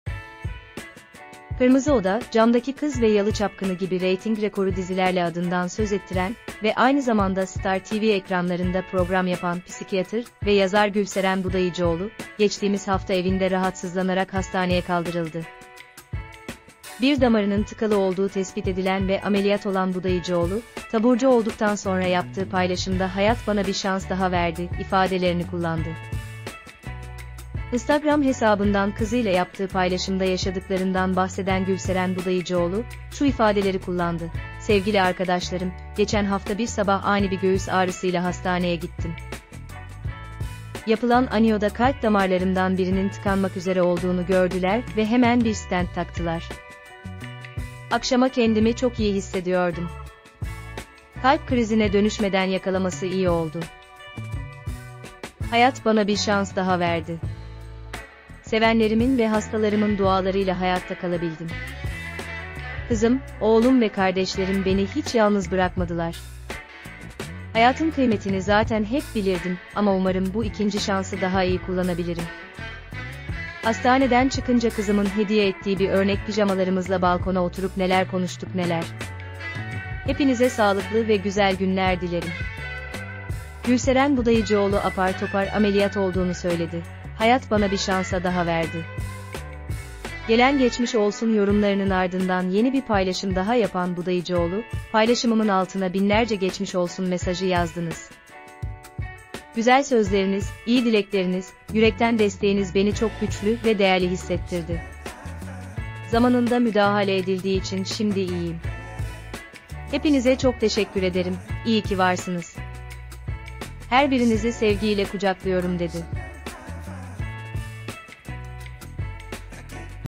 Gülseren Budayıcıoğlu apar topar ameliyat olduğunu söyledi: Hayat bana bir şansa daha verdi
Kırmızı Oda, Camdaki Kız ve Yalı Çapkını gibi reyting rekoru dizilerle adından söz ettiren ve aynı zamanda Star TV ekranlarında program yapan psikiyatr ve yazar Gülseren Budayıcıoğlu, geçtiğimiz hafta evinde rahatsızlanarak hastaneye kaldırıldı. Bir damarının tıkalı olduğu tespit edilen ve ameliyat olan Budayıcıoğlu, taburcu olduktan sonra yaptığı paylaşımda Hayat bana bir şans daha verdi" ifadelerini kullandı. Instagram hesabından kızıyla yaptığı paylaşımda yaşadıklarından bahseden Gülseren Budayıcıoğlu, şu ifadeleri kullandı: Sevgili Arkadaşlarım, geçen hafta bir sabah ani bir göğüs ağrısıyla hastaneye gittim. Yapılan anjiyoda kalp damarlarımdan birinin tıkanmak üzere olduğunu gördüler ve hemen bir stent taktılar. Akşama kendimi çok iyi hissediyordum. Kalp krizine dönüşmeden yakalaması iyi oldu. Hayat bana bir şans daha verdi. Sevenlerimin ve hastalarımın dualarıyla hayatta kalabildim. Kızım, oğlum ve kardeşlerim beni hiç yalnız bırakmadılar. Hayatın kıymetini zaten hep bilirdim ama umarım bu ikinci şansı daha iyi kullanabilirim. Hastaneden çıkınca kızımın hediye ettiği bir örnek pijamalarımızla balkona oturup neler konuştuk neler… Hepinize sağlıklı ve güzel günler dilerim.Gülseren Budayıcıoğlu apar topar ameliyat olduğunu söyledi: Hayat bana bir şansa daha verdi.Gelen geçmiş olsun yorumlarının ardından yeni bir paylaşım daha yapan Budayıcıoğlu, Paylaşımımın altına binlerce geçmiş olsun mesajı yazdınız. Güzel sözleriniz, iyi dilekleriniz, yürekten desteğiniz beni çok güçlü ve değerli hissettirdi. Zamanında müdahale edildiği için şimdi iyiyim. Hepinize çok teşekkür ederim, iyi ki varsınız... Her birinizi sevgiyle kucaklıyorum dedi.